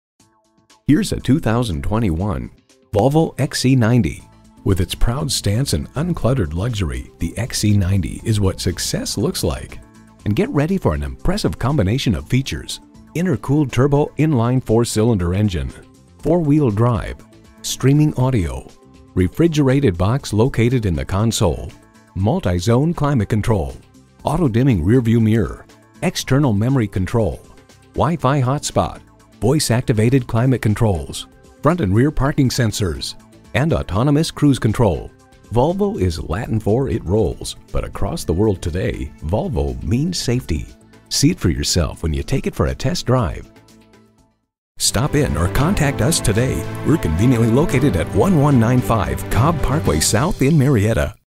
Used 2021 Volvo XC90 Marietta, GA Atlanta, GA Q12442 - SOLD
Year: 2021
Make: Volvo
Model: XC90
Trim: T5 Momentum
Engine: I4
Transmission: Automatic with Geartronic
Color: White
Interior: Blond
Mileage: 26545
Stock #: Q12442
VIN: YV4102PK6M1725560

Address:
1195 Cobb Parkway S